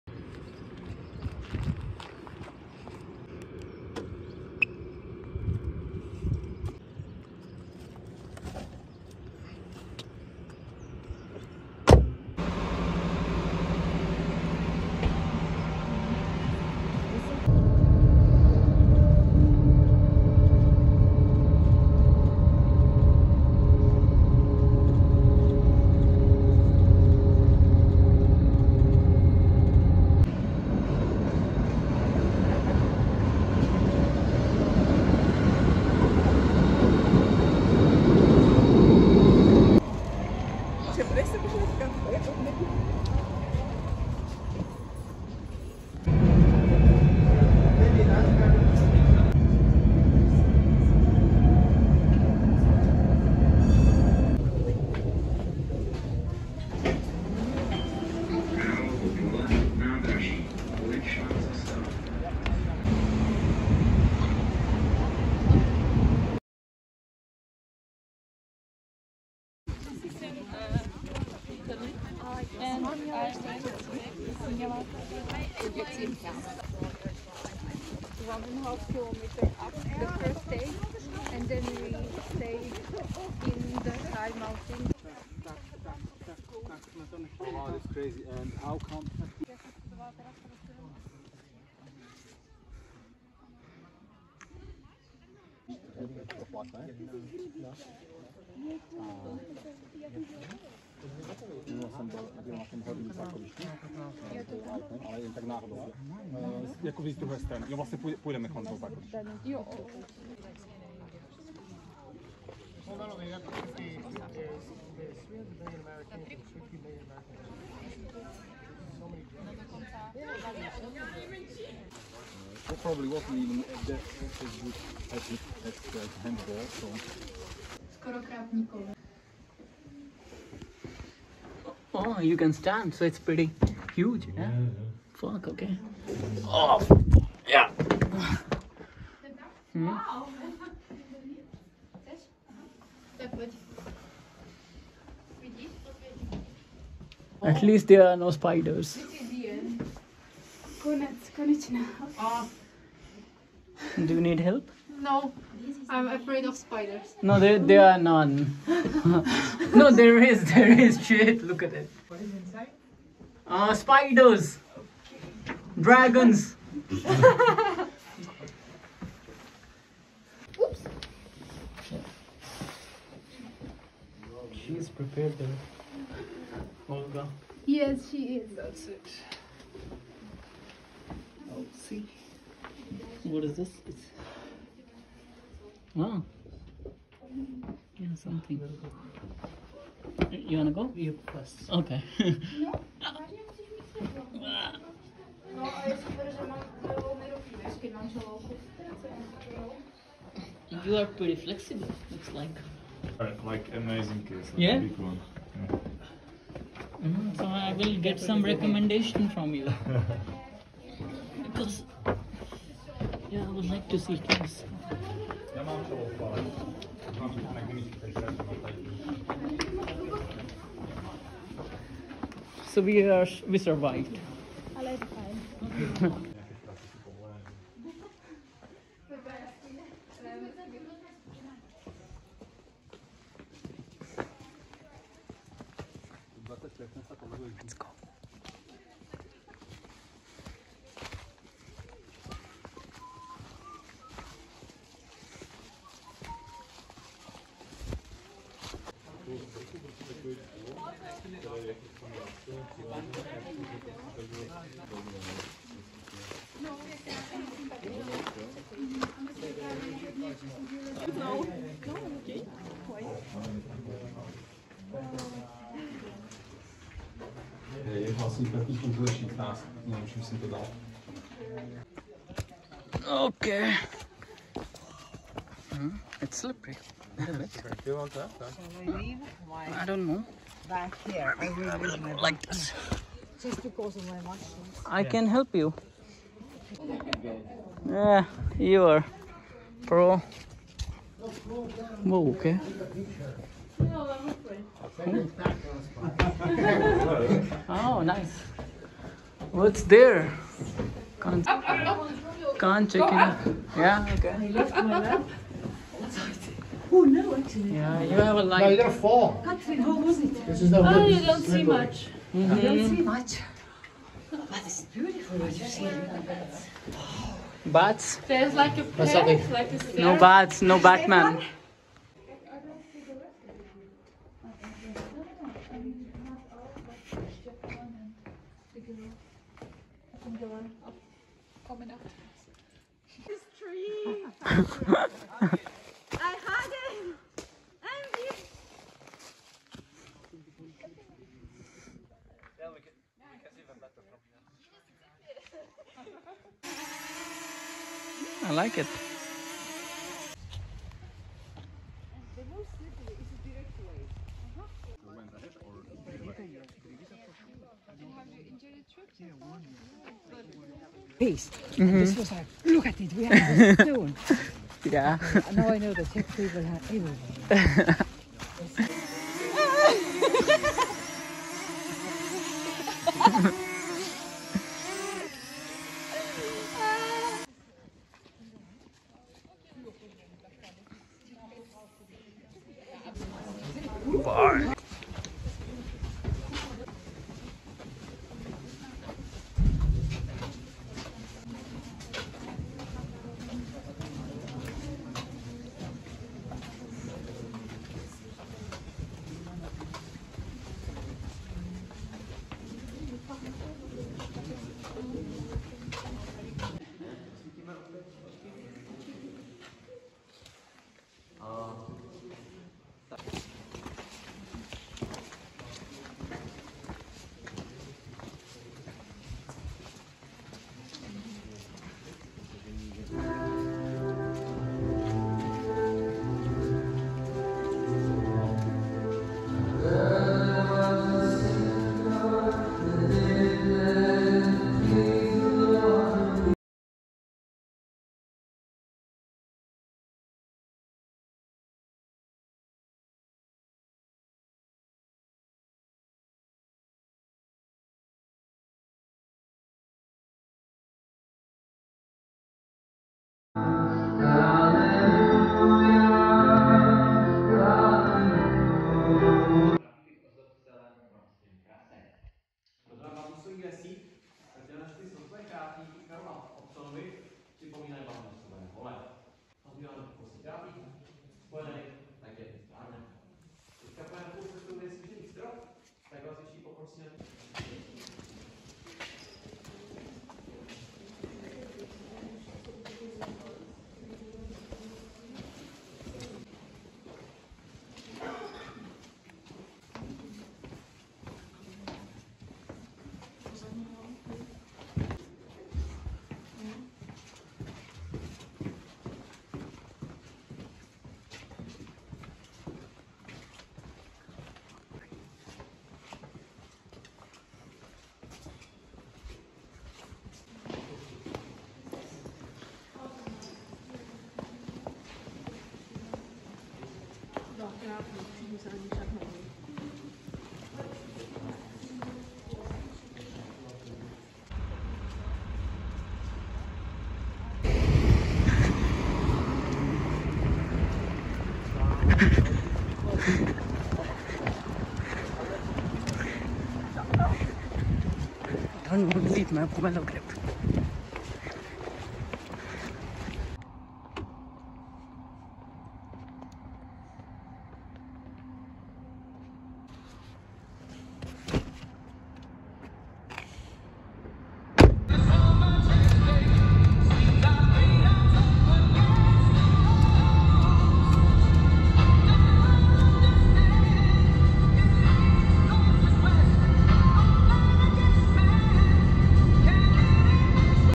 Trip to South Moravian Caves
This is just a simple video of my day trip to the Caves in south Moravia. In the end i tried to share things which are essential while visiting caves.